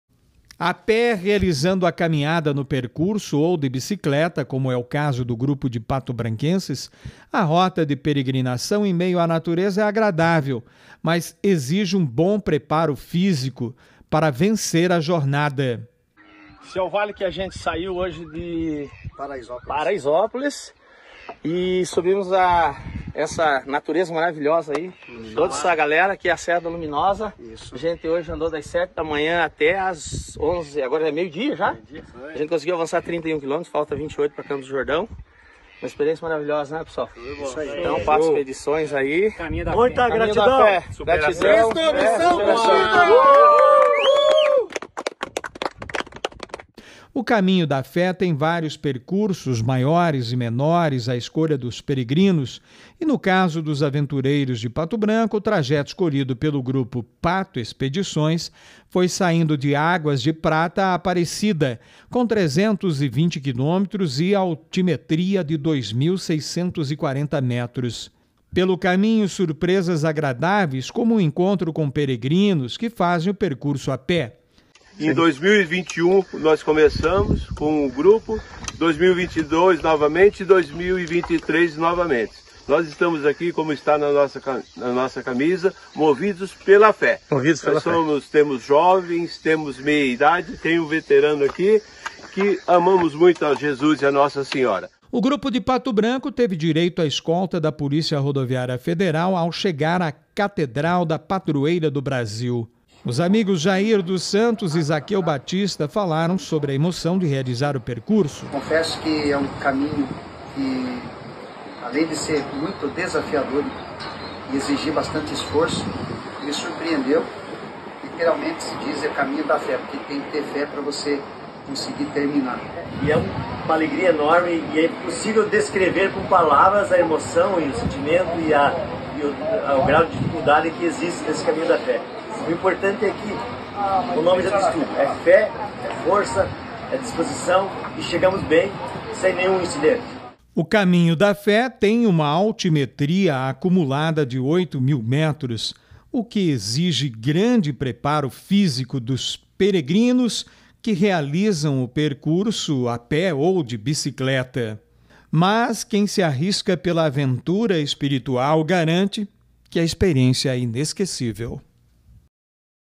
Grupo de ciclistas de Pato Branco realiza o percurso Caminhos da Fé
Religião: O “Caminho da Fé” no Brasil é inspirado no percurso de 800 quilômetros, famoso no mundo inteiro e atraindo no turismo religioso milhares de pessoas. Aqui no Brasil, o nosso “Caminho da Fé” também atrai milhares de turistas todos os anos. Acompanhe um pouco do que foi esta aventura do grupo patobranquense, através da colaboração de Ivan Orlandini que integra a equipe Pato Expedições.